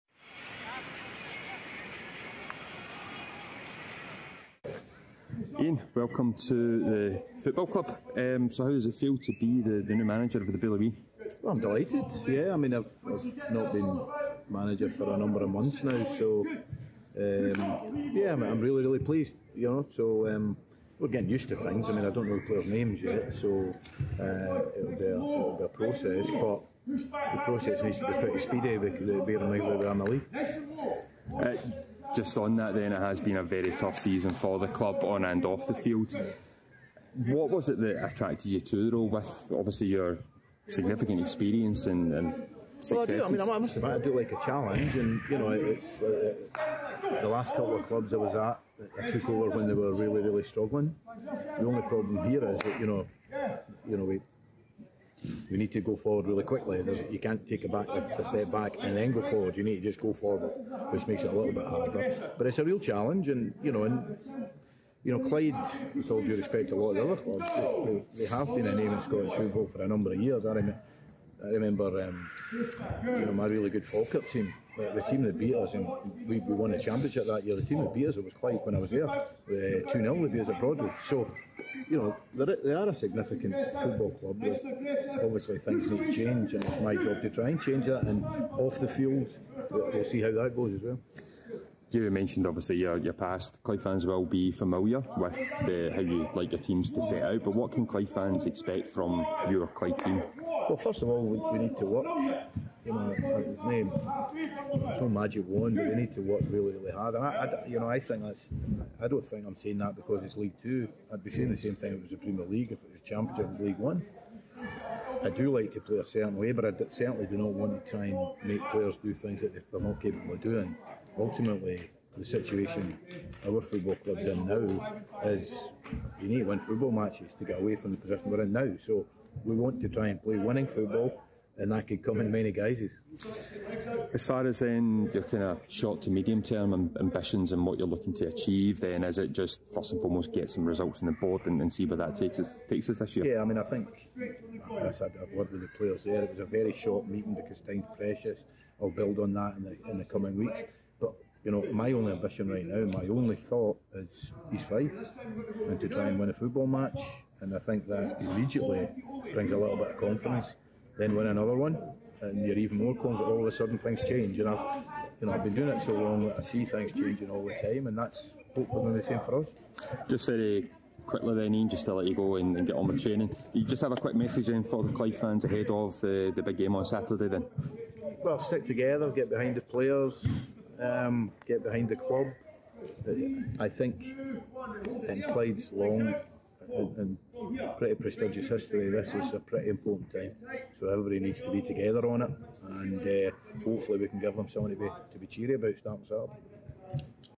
First Interview with New Manager Ian McCall
Ian McCall gives his first interview as Clyde boss as he states what attracted him to the role and his aims going forward.

**Apologies for our enthusiastic Sports Scientist in the background**